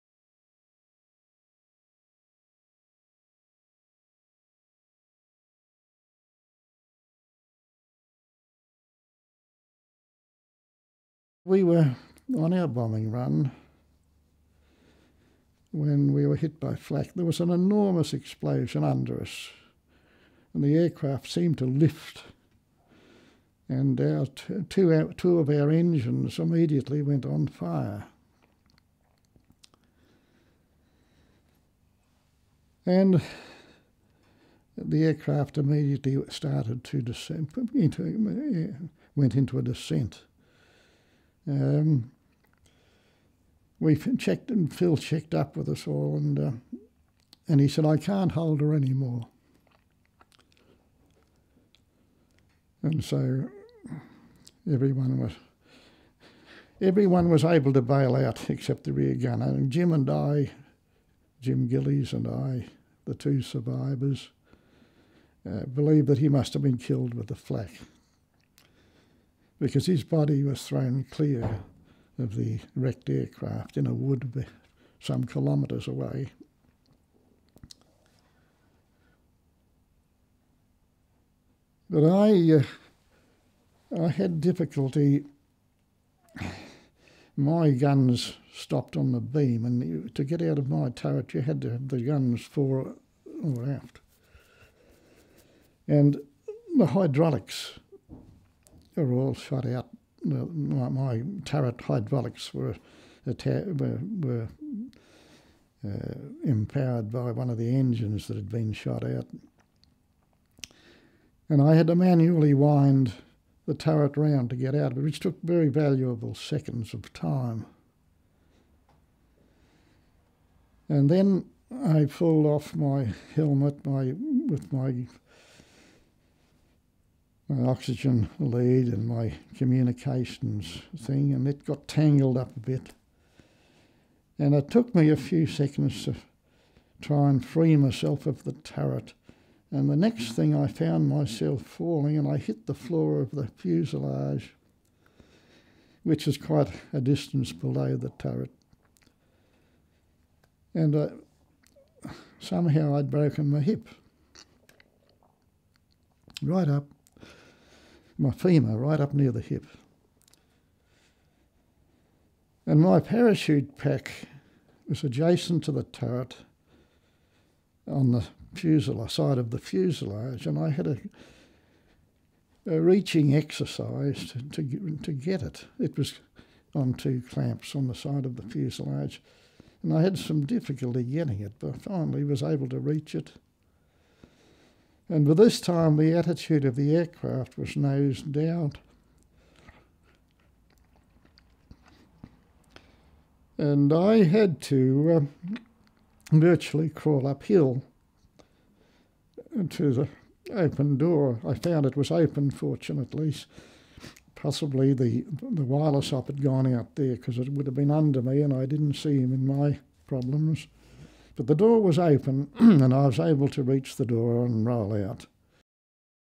BAWDEN Harvey
BAWDEN Harvey 419835
Harvey Bawden was a mid-upper gunner in a Lancaster with 150 Squadron RAF.
On a daylight raid on a German synthetic oil plant his Lancaster was shot down by flak. Most of the remaining aircrew were been murdered by the Germans after they landed. Harvey was one of only two survivors. He tells the story of how he managed to leave the stricken aircraft.